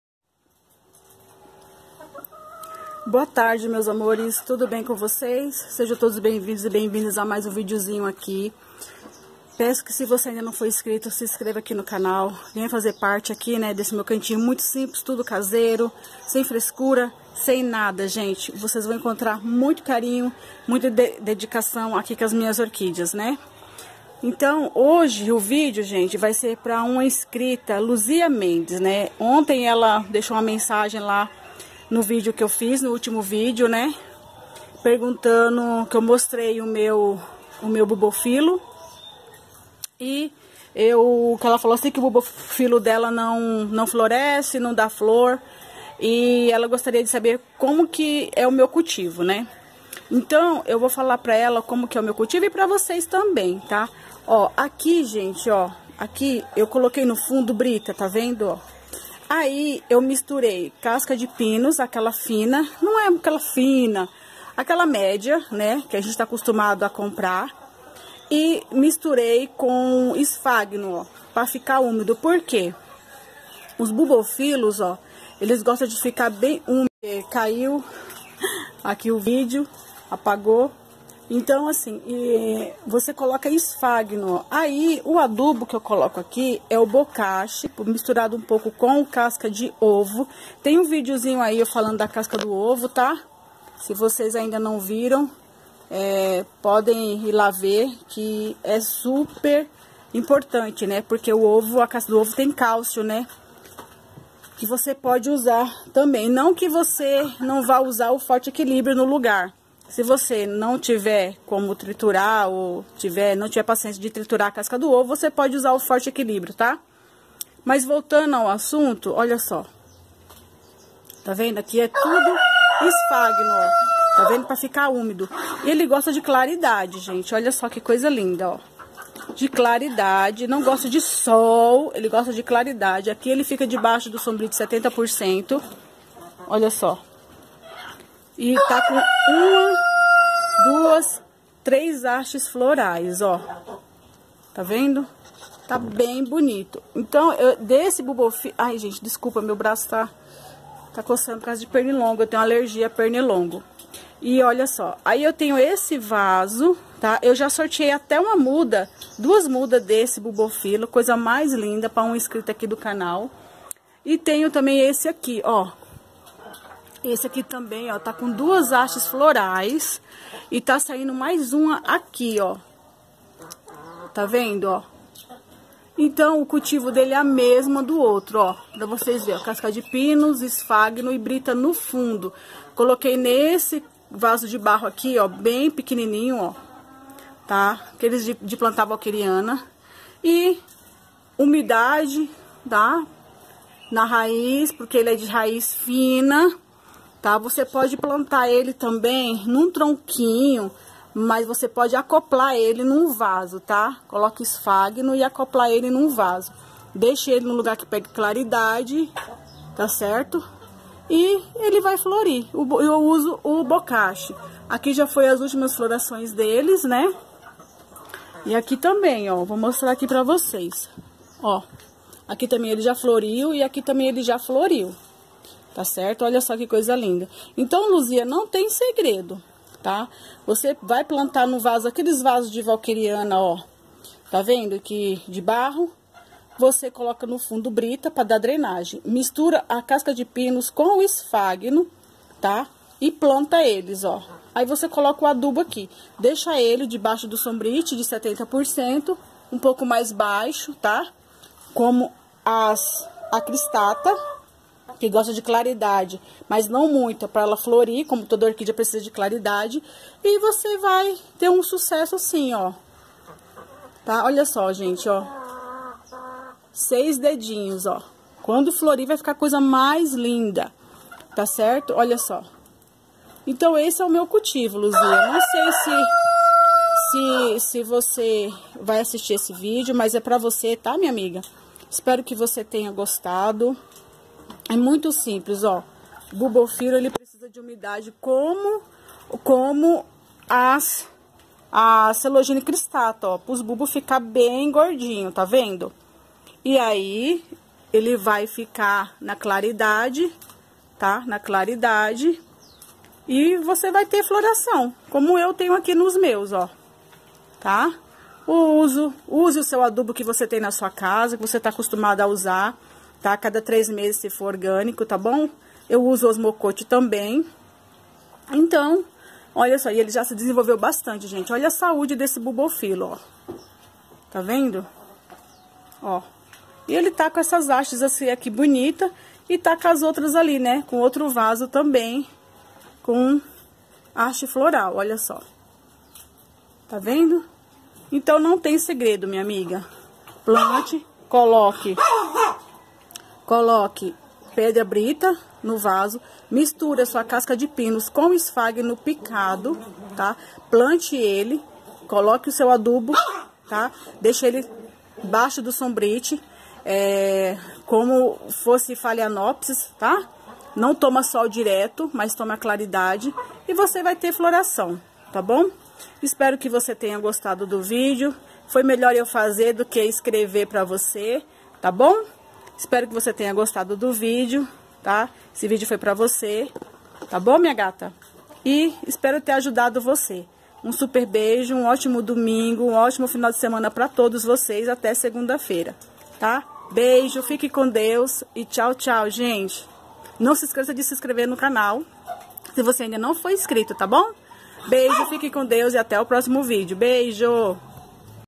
BULBOFILON a pedido de uma inscrita 😍
Boa noite meus amores hoje o vídeo respondendo a uma inscrita muito querida espero que ela goste e vcs também ,um super beijo e fiquem todos com Deus 🙏

ATÉ AQUI DEUS ME SUSTENTOU 🙏